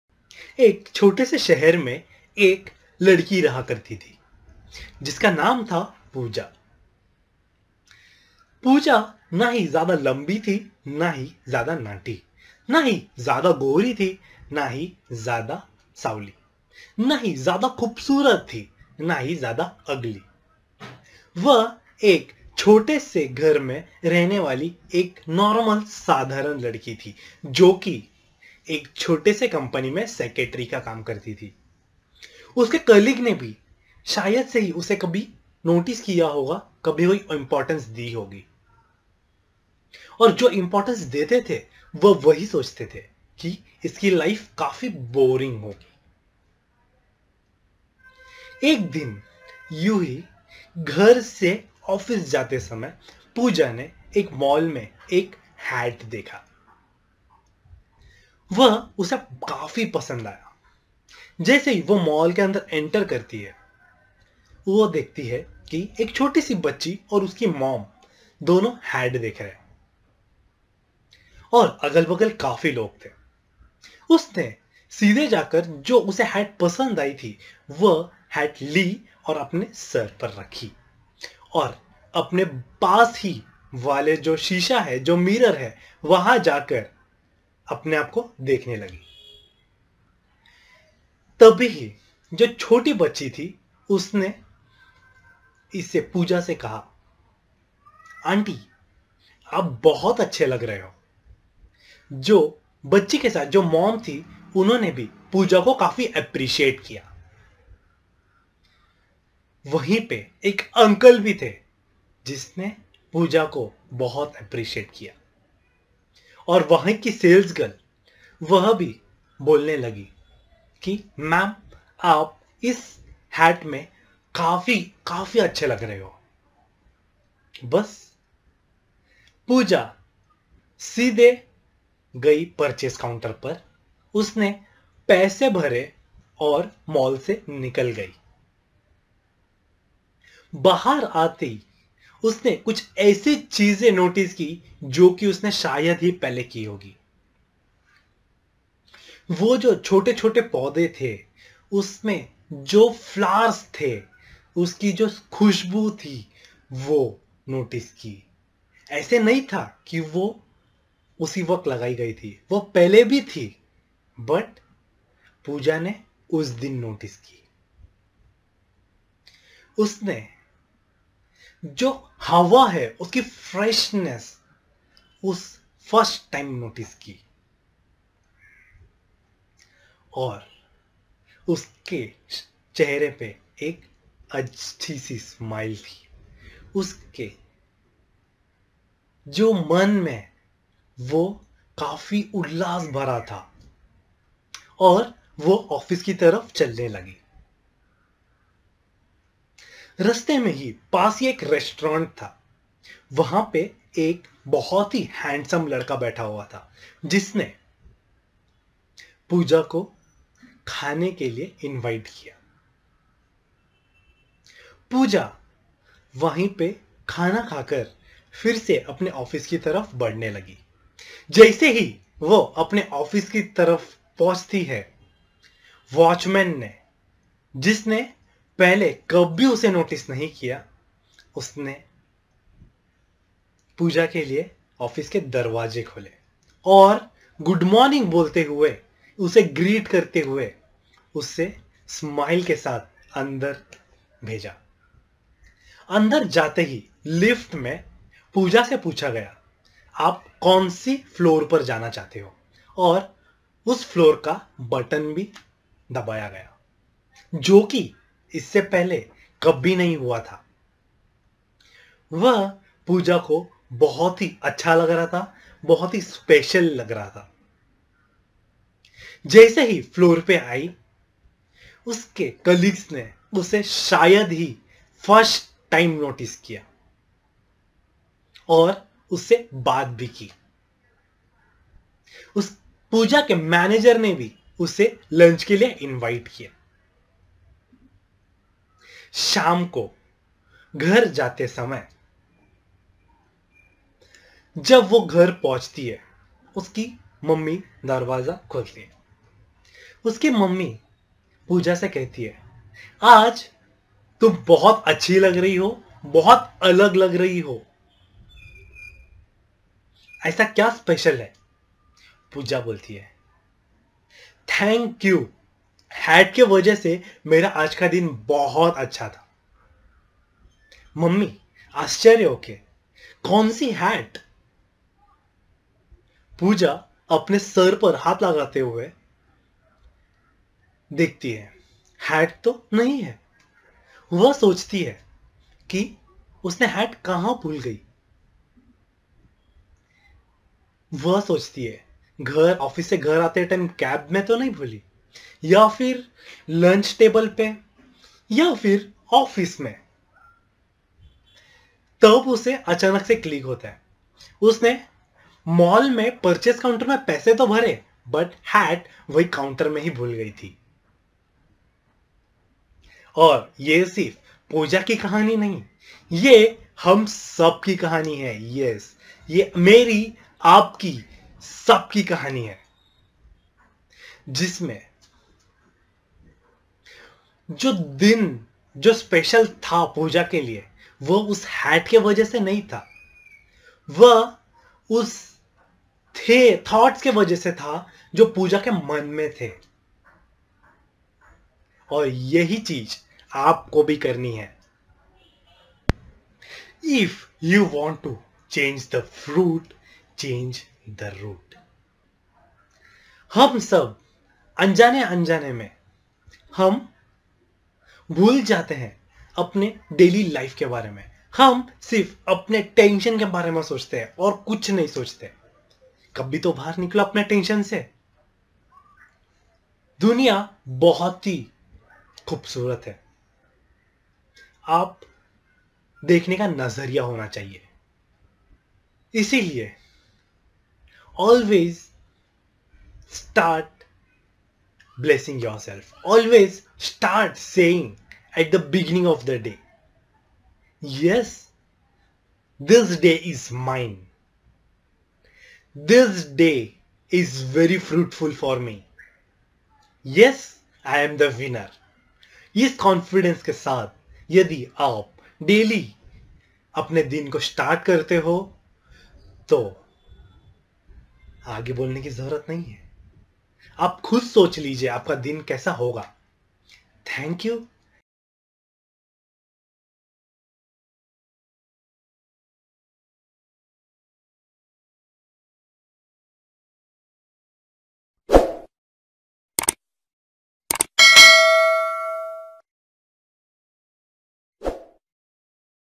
Change your Boring Day in to Productivity Day
We usually tends to get bored with daily task from morning to evening.
This video tells you, a change in single thought will change your complete day.